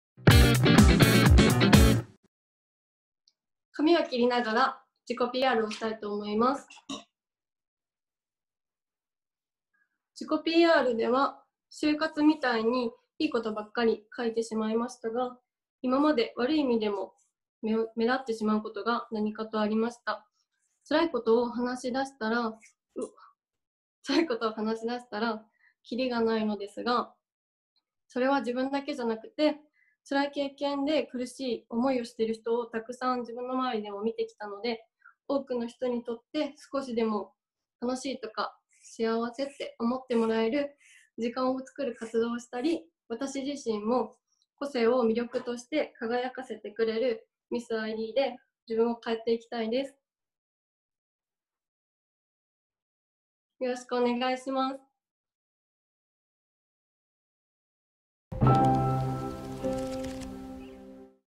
ミスiD2021 ひかり
＊新型コロナウイルス感染症拡大防止のため、今年はzoomを使って、遠隔でのカメラテスト（自己PR動画撮影）を実施しました。
それぞれの通信環境などにより、画質や音質のばらつき、まれに映像の乱れがございます。何卒ご了承ください。